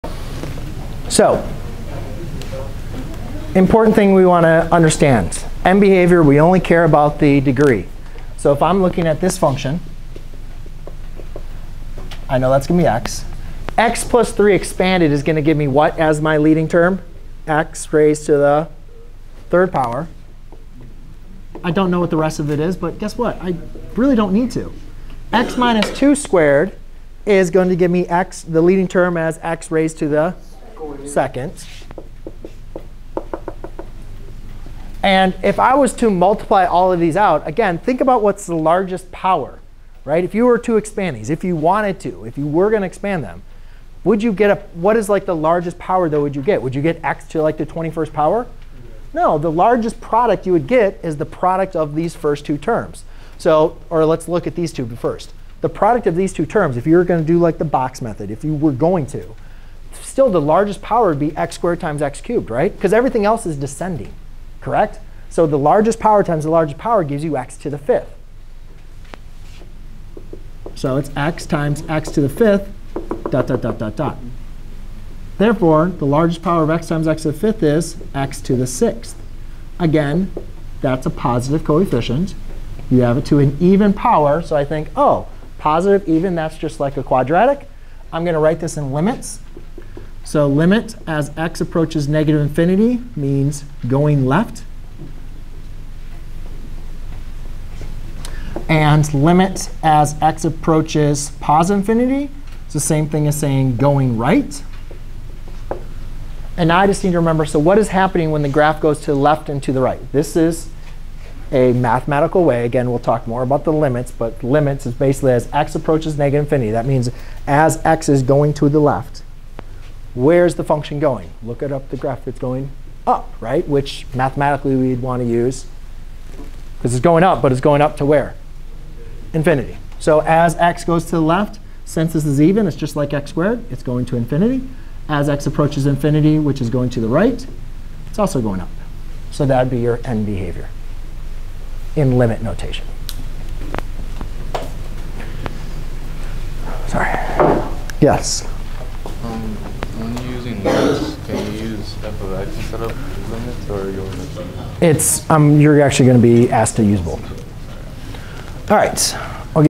How to determine the end behavior from the product of factors
👉 Learn how to determine the end behavior of the graph of a factored polynomial function. To do this we will first need to make sure we have a polynomial in standard form (i.e. we will expand all factored terms) with descending powers. We will then identify the leading terms so that we can identify the leading coefficient and degree of the polynomial.